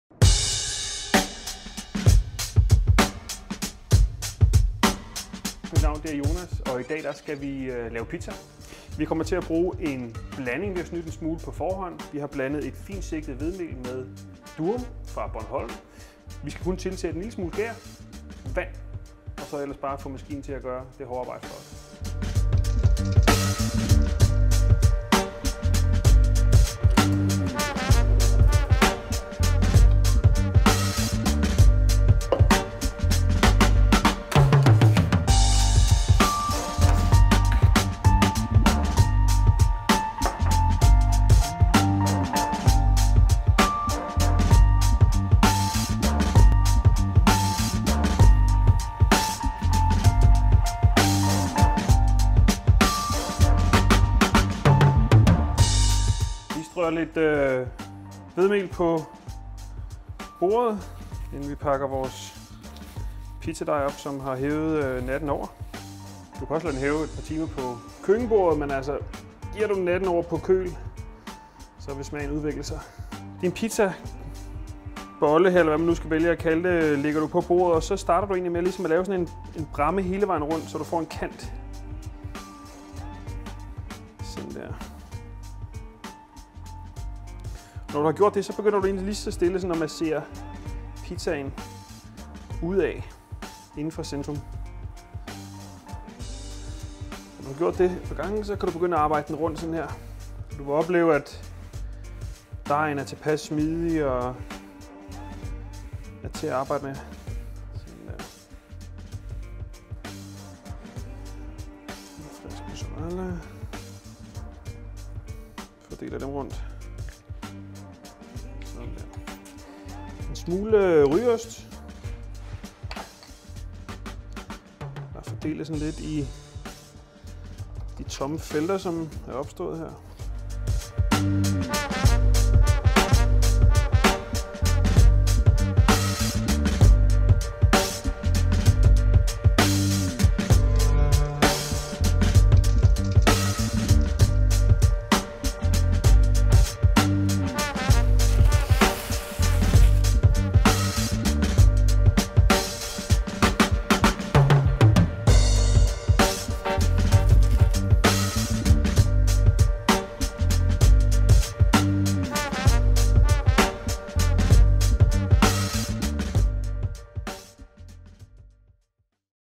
Sådan laver du pizza med Meyers blanding (fulde version)
Godt mel er udgangspunktet for en god pizzabund, og derfor har vi brugt Meyers finsigtede Type 00 hvedemel og den bornholmske durummel, som sammen med tørsurdej og salt bliver til vores nye pizzablanding.

Hemmeligheden bag Meyers himmelske sprøde pizzaer er ikke så meget dejens tilblivelse, som måden du strækker den udover et stykke linned foldet om en skål. Teknikken giver dig mulighed for at arbejde med en ekstremt tynd dej.

Se med i videoen og få inspiration fra vores produktudvikler Jonas, der viser, hvordan du laver en appetitvækkende pizza med vores pizzablanding til weekenden.

BAGETIP: Har du travlt kan du fordoble gærmængden, og i stedet lade dejen hæve 2-3 timer på køkkenbordet.

Find vores pizzablanding hos Kvickly, Super Brugsen og hos coop.dk